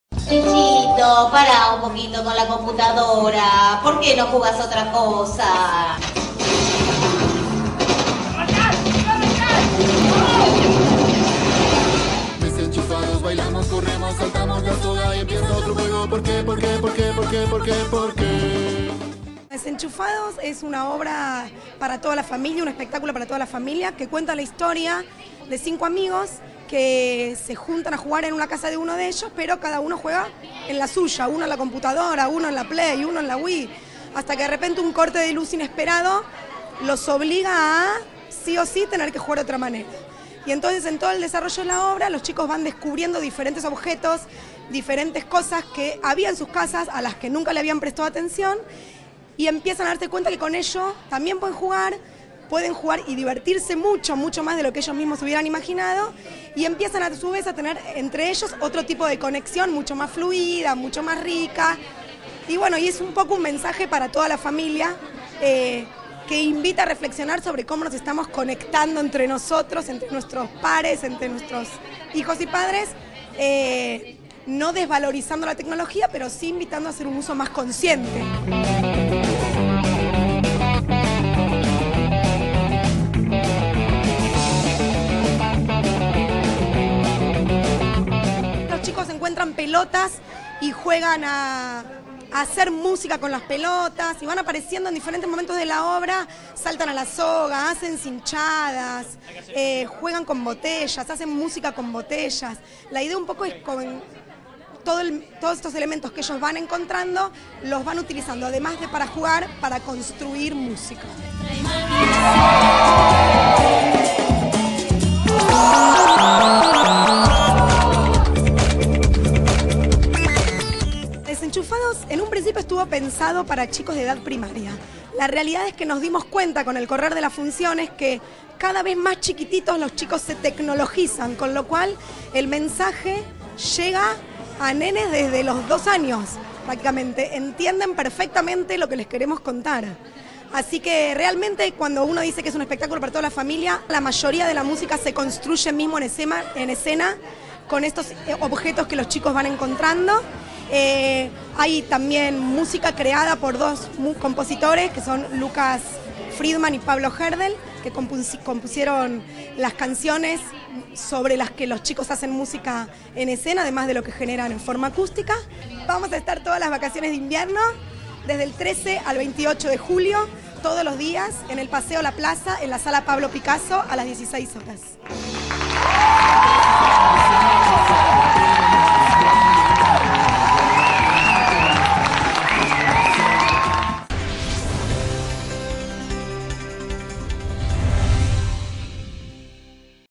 "Desenchufados": un imprevisto corte de luz da pie a juegos creativos con tachos, palos y pelotas
La obra "Desenchufados" se construye a través de la creación de músicas y coreografías, cuando por imperiosa necesidad de superar el aburrimiento que parece terminal, los chicos comienzan a usar creativamente elementos de uso cotidiano y otros de desecho: tachos de basura, palos, pelotas, sogas, linternas, escobas y el propio cuerpo.

"Desenchufados" cuenta la historia de un grupo de chicos que inicialmente sólo se relacionan entre sí a través de la tecnología. Uno de ellos invita a sus amigos a jugar a su casa y, a medida que van llegando, cada uno toma un elemento tecnológico: videojuego, consolas, computadora, celular.

Pero, imprevistamente, se corta la luz. A partir de allí, comienzan a pasar del aburrimiento a la exploración. Y empiezan a encontrar de qué otra manera pueden jugar. Descubren un patio trasero que les aporta ciertos elementos que despiertan su creatividad y ganas de divertirse. La obra se construye a través de la creación de músicas y coreografías utilizando elementos de uso cotidiano y otros de desecho como tachos de basura, palos, pelotas, sogas, linternas, escobas y el propio cuerpo.

En la Sala Pablo Picasso del Paseo La Plaza se presenta este creativo musical escrito y dirigido por Ariadna Faerstein y Flor Yadid, todos los días de las vacaciones de invierno a las 16 horas. Una propuesta para toda la familia que combina percusión, música y humor.